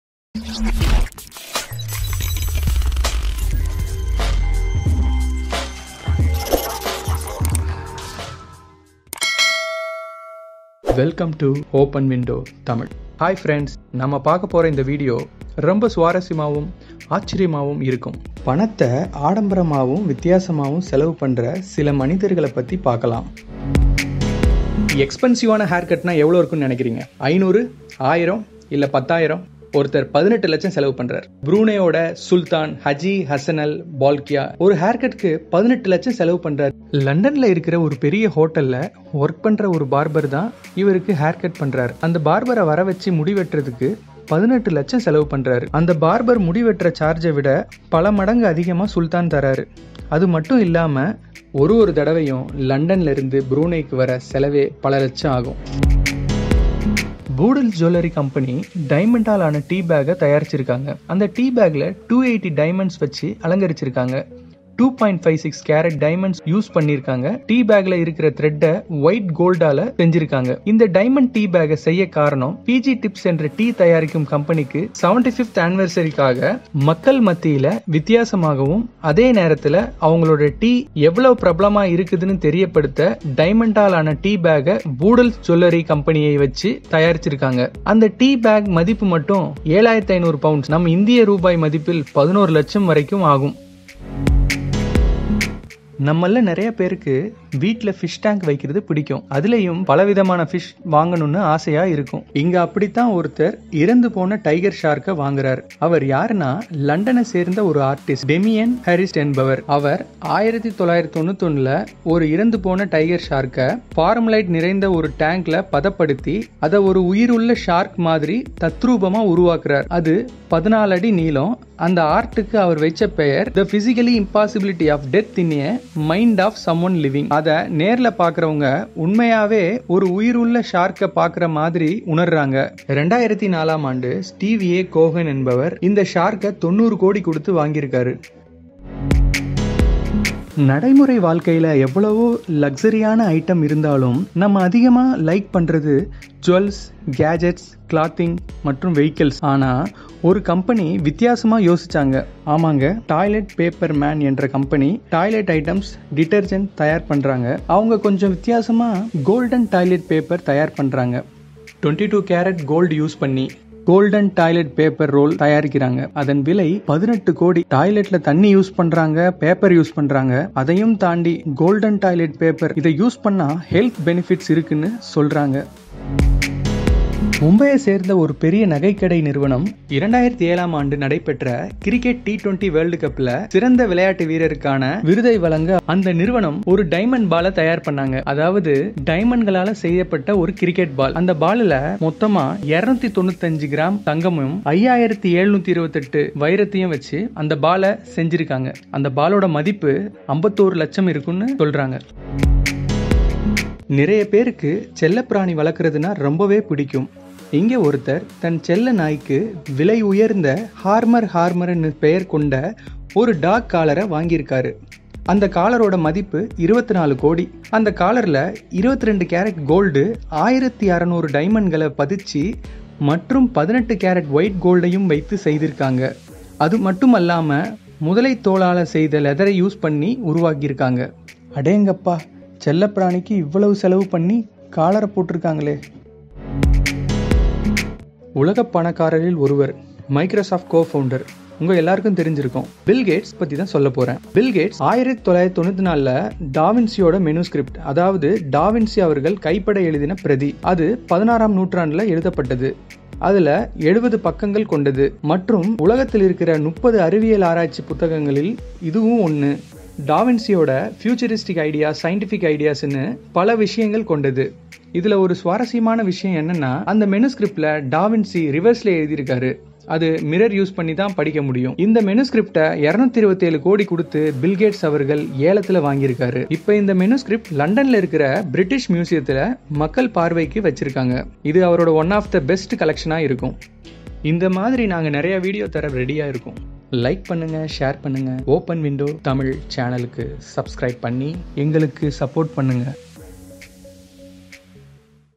MOST EXPENSIVE THINGS | விநோதமாவும் விசித்திரமாகவும் விலை உயர்ந்த சில தகவல்கள் | OPEN WINDOW TAMIL
Hi Friends,

Welcome to OPEN WINDOW TAMIL

This video is based on MOST EXPENSIVE THINGS , விநோதமாவும் விசித்திரமாகவும் விலை உயர்ந்த சில தகவல்கள்

we share infinite information about unknown facts, history, Mysterious things, Inventions, Technology, etc,...
 1. EXPENSIVE HAIR CUT:
                  Haji Hassanal Bolkiah, the Sultan of Brunei Sultan of Brunei, dropped more than $24,000 on his most recent trim, the Times
                  Haji Hassanal Bolkiah, the Sultan of Brunei Sultan of Brunei, dropped more than $24,000 on his most recent trim,
                  The 63-year-old Sultan – one of the richest men in the world – pays the barber thousands every time he makes the 7,000 mile round trip to Brune.
                   The barbar has been cutting the hair of the sultan for 16 years..

2. EXPENSIVE DIAMOND TEA BAG:
                          PG Tips is the British company on tea manufacturing
                         Jeweler company Boodles made this diamond tea bag in honor of the 75th anniversary of the PG Tips. It was handmade and decorated with 280 diamonds, which explains the price tag of $ 14,000.
                         Well, we all know that the British are serious about their favorite drink, but not so!

  3.EXPENSIVE DEAD SHARK:
                          In 2004, billionaire hedge fund manager Steve Cohen bought what just might be the world’s most unique piece of contemporary art: a 14-foot tiger shark preserved with a combination of alcohol and formaldehyde.
                         The dead shark is an art-piece which is nothing more than a stuffed tiger shark in a tank full of formaldehyde. NY Post attribute the sharks high price tag to branding and the fact that it was commissioned .
                          The piece is actually a 1991 creation by Damien Hirst. It is officially called “The Physical Impossibility of Death in the Mind of Someone Living” and is the kind of item that could certainly cause nightmares.
                           Hirst used the carcass of a tiger shark, preserved in formaldehyde to create this terrifying image. The current owner of the 12 million dollar “Dead Shark”

4.EXPENSIVE GOLD TOILET PAPER:
                           Fancy throwing your money straight down the toilet? Then you need to own the most expensive toilet paper in the world.
                           Have you ever wondered what a super rich person household shopping list may contain? Besides very expensive food, how about a 22-carat gold toilet paper?
                           The thing is, some millionaires have taken that one step further, and started buying toilet paper made from real gold.

5. EXPENSIVE  DIAMOND CRICKET BALL
                           The diamond cricket ball was created by Gitanjali Gems Limited.
                          This diamond cricket ball was presented to the Best Indian and Best International players at the 2007 Cricket World Cup tournament.
                          The balls are rounded out with 5,728 diamonds with an 18-carat gold.

6. EXPENSIVE DIAMOND DOG COLLAR:
                        Everyone loves their dog – they’re “man’s best friend” after-all. However, no matter how much money I had to spend, I’m not sure I’d ever be willing to drop 3.2 million dollars on a dog collar.
                     The Amour Amour dog collar is 52 carats and encrusted with 1,600 individual diamonds.

 7.EXPENSIVE MANUSCRIPT:
                     Bill Gates’ Scientific Scribbles by Leonardo Da Vinci
                     In 1994, Gates spent $30.8 million to own the Codex Leicester, a 72-page manuscript that da Vinci compiled in the early 16th century, complete with the master polymath’s diagrams, writings, sketches and ideas for future inventions.
piled in the early 16th century, complete with the master polymath’s diagrams, writings, sketches and ideas for future inventions.

support us for a long.